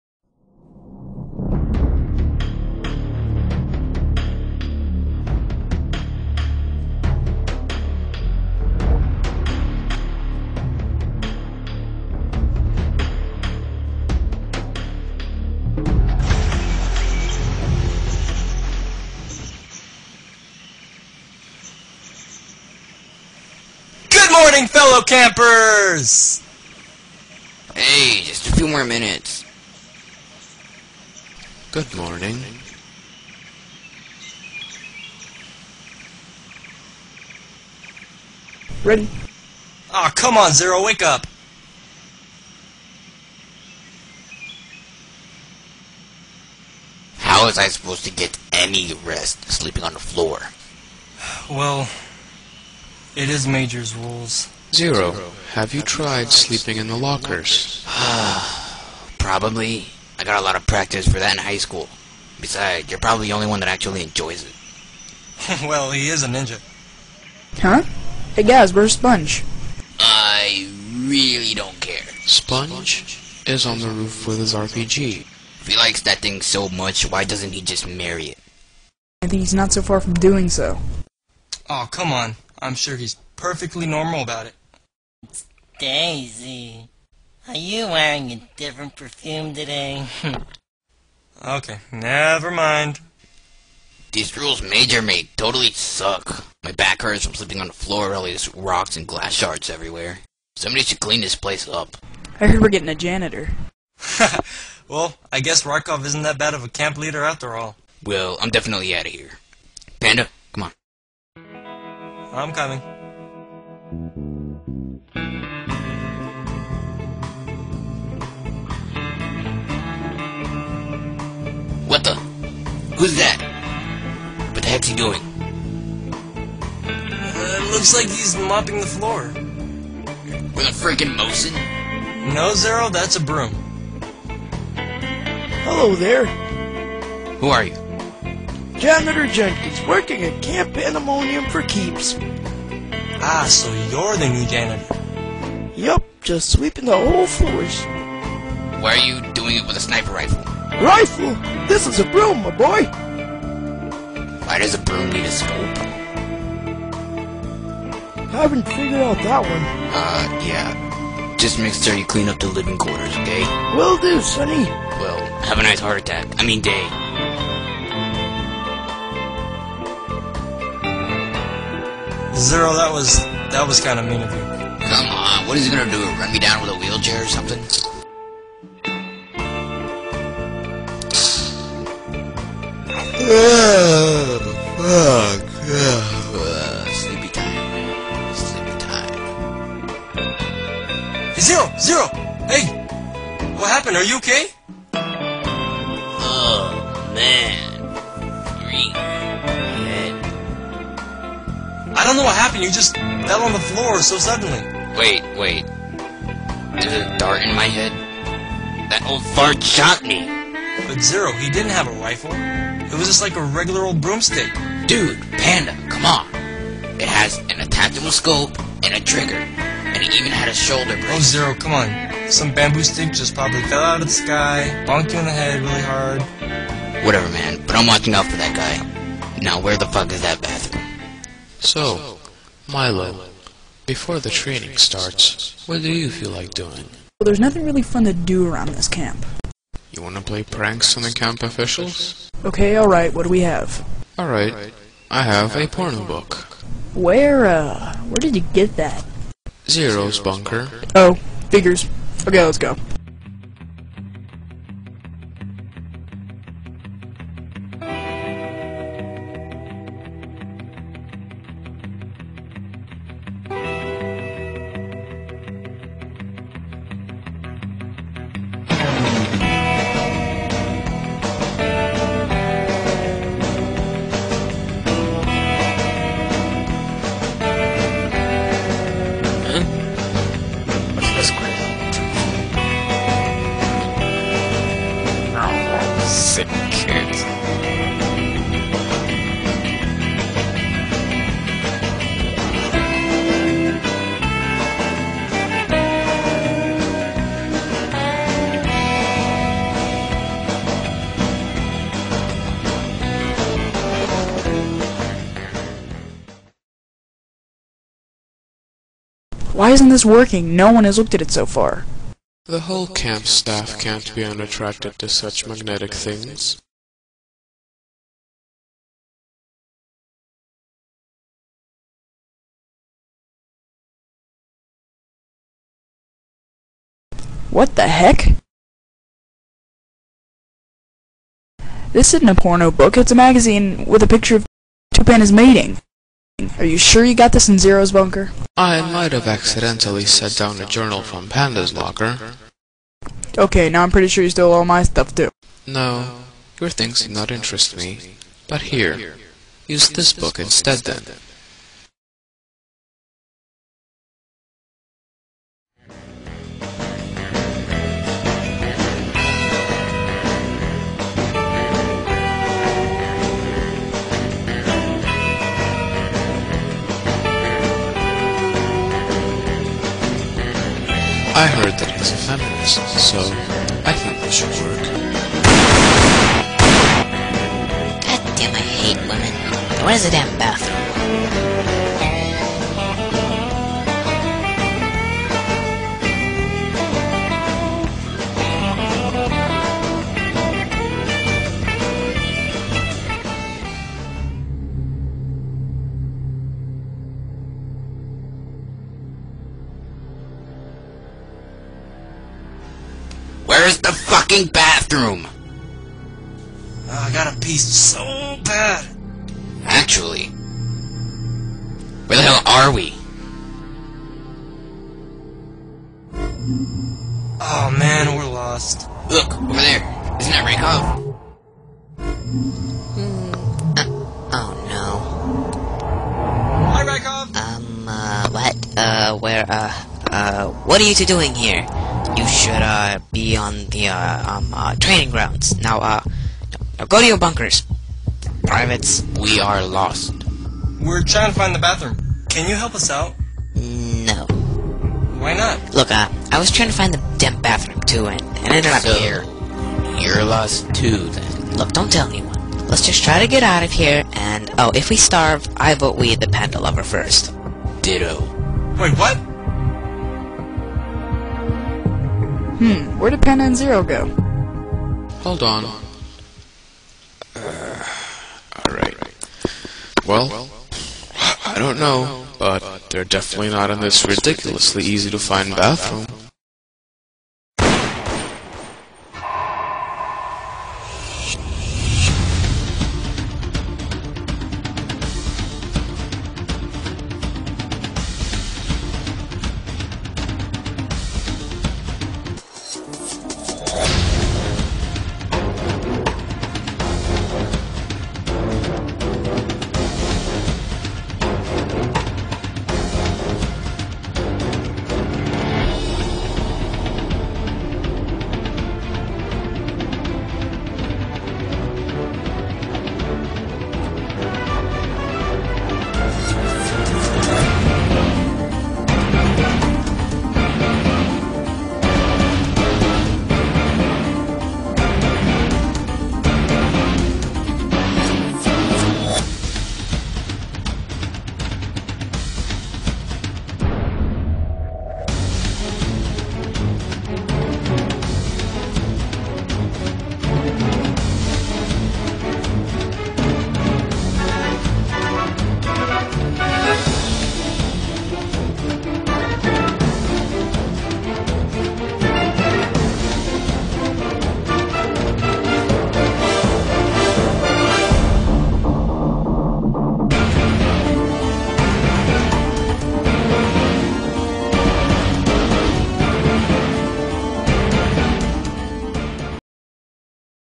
Camp Panda Ops: Episode 2
Camp PandaMonium, a camp for all the lost soldiers, trained to become the best, have a place in the world, and discover things beyond what their eyes could ever see!

But...not for these guys. Getting lost by trying to find the bathroom. Playing tricks on your fellow Campers, sleeping under the bed instead of on it, and. love blooms on the battlefield.